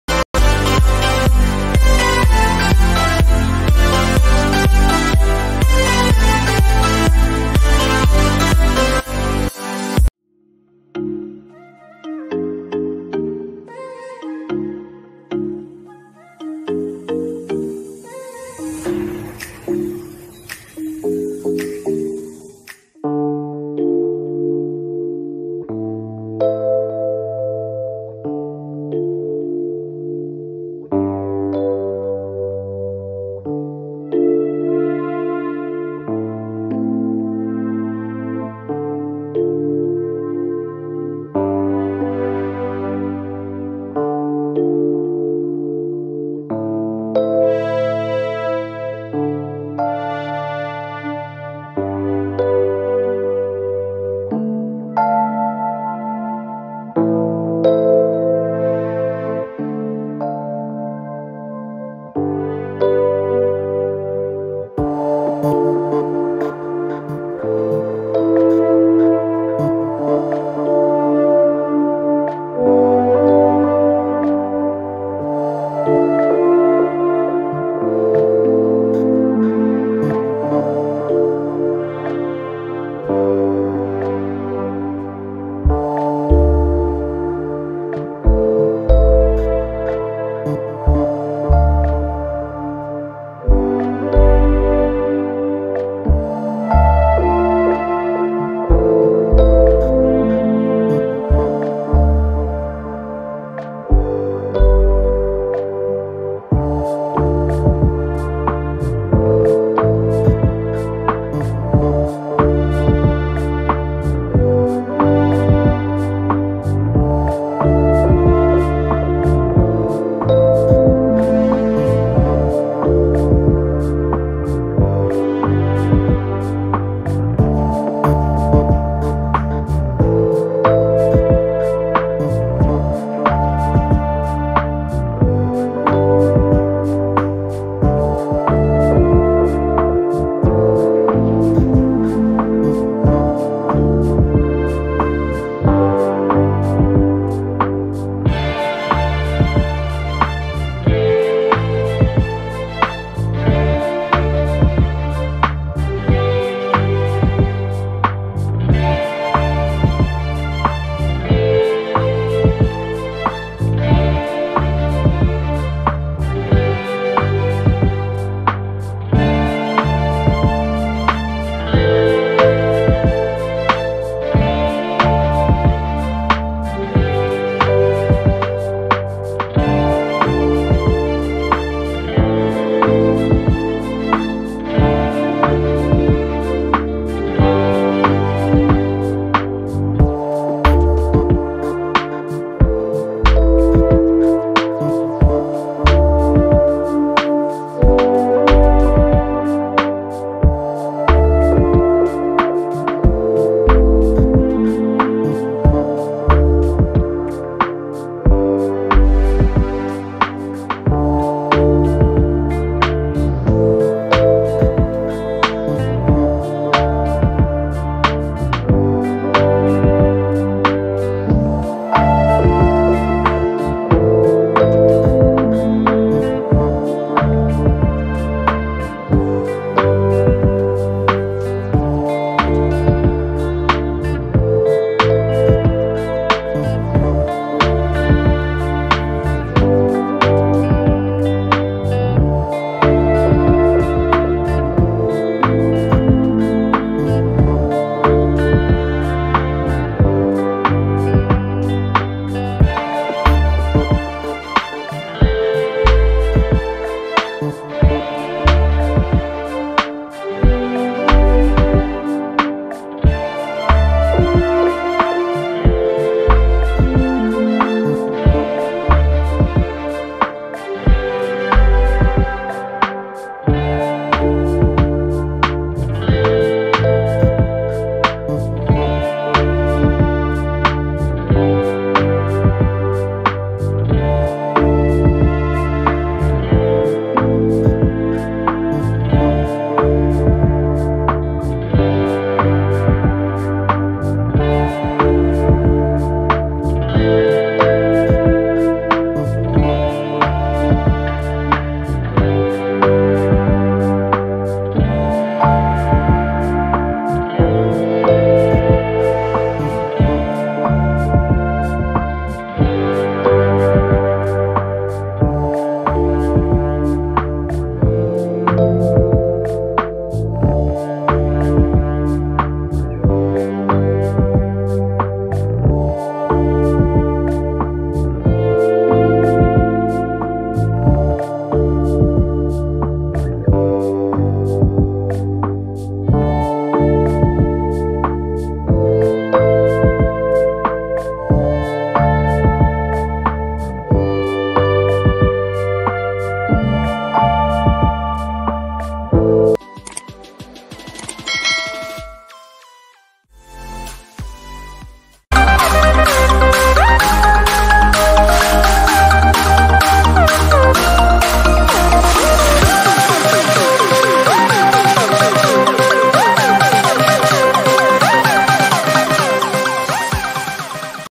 Happy Diwali drawing very easy | Happy Diwali 2021 drawing using oil pastel
In this video, I Tell you how to draw Diwali drawing for beginner Step by Step using oil pastel.